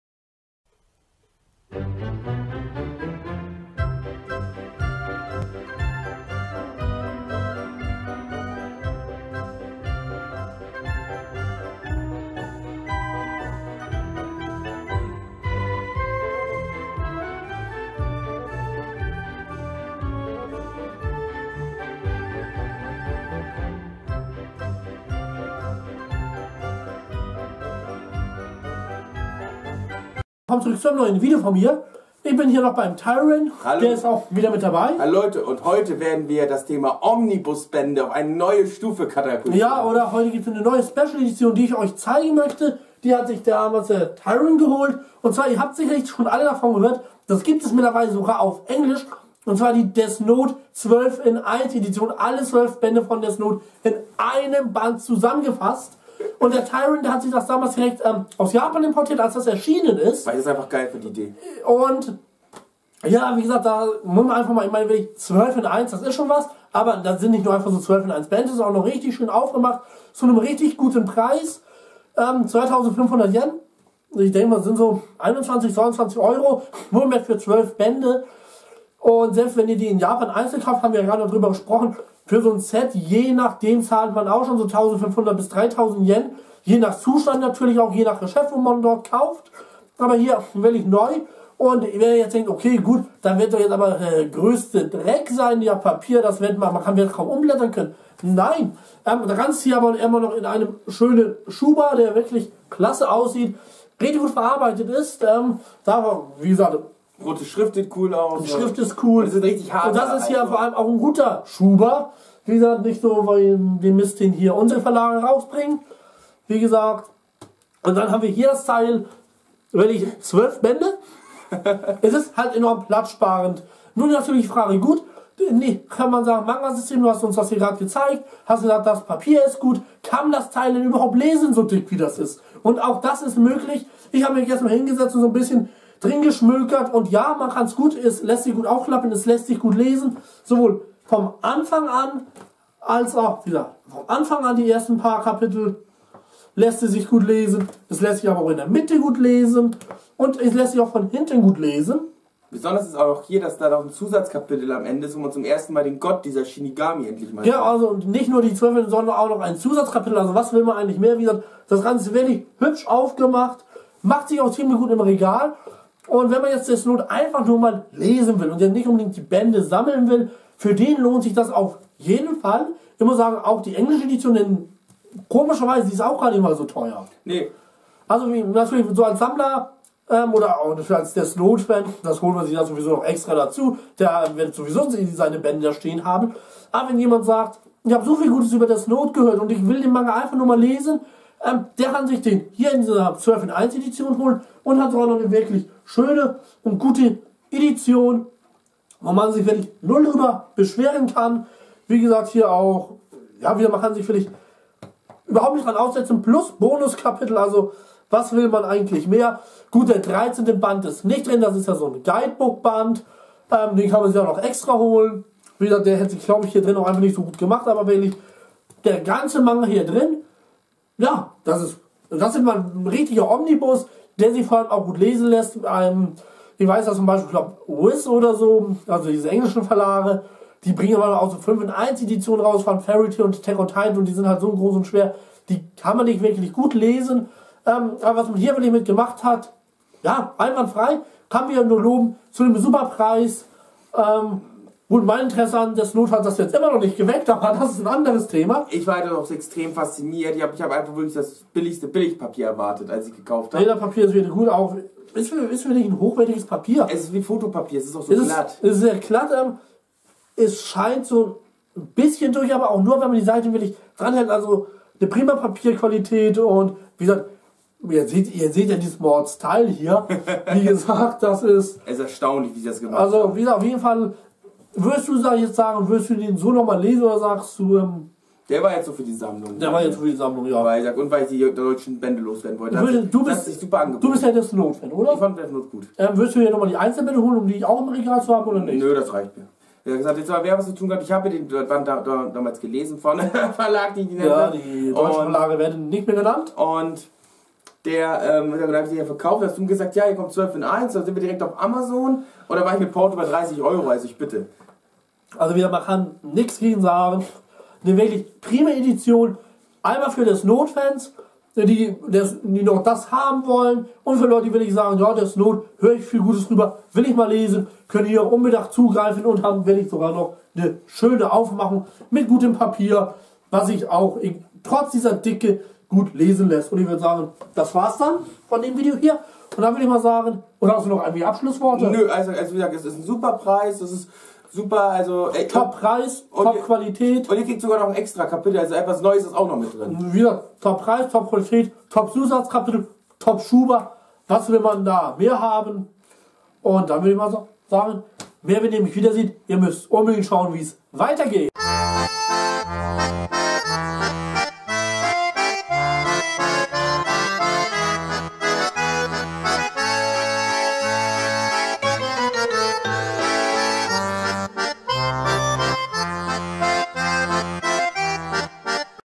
Death Note 12 in 1 Edition
Heute zeige ich euch die 12-1 Edition von Death Note mit dabei der Tyrann.
(Es wird noch urz ein anderes Video erwähnt was ich vor ein paar Wochen aufgenommen habe dies kommt wenn alle Videos mit dem Tyrann hochgeladen sind)